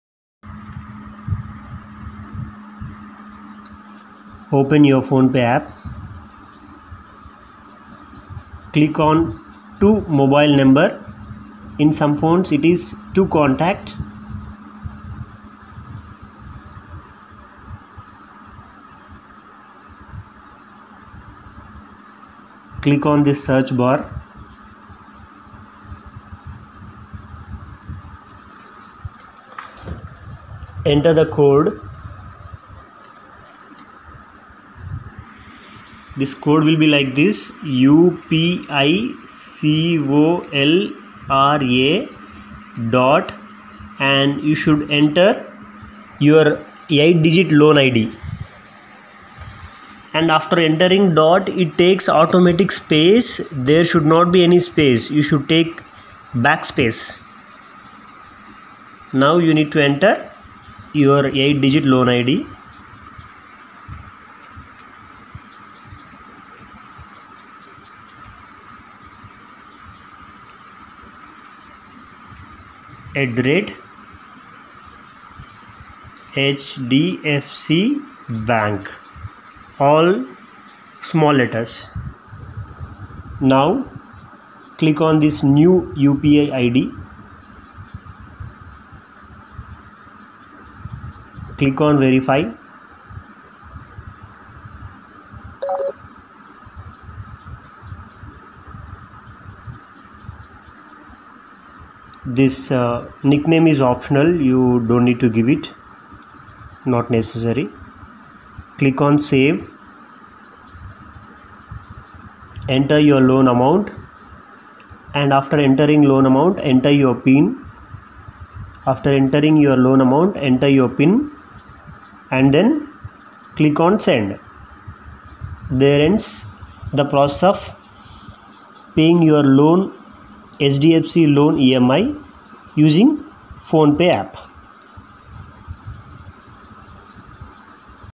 How to pay hdfc Loan emi using phonepe app?
Melange of knowledge
1. Open your phonepe app
2. Click on To mobile no. / To Contact
3. Click on search bar.
4. Enter the Code
(upicolra.your8digitloanid@hdfcbank
5. Click on new upi
6. Click on verify
7. Click on Save
8. Enter your pin
9. Click on send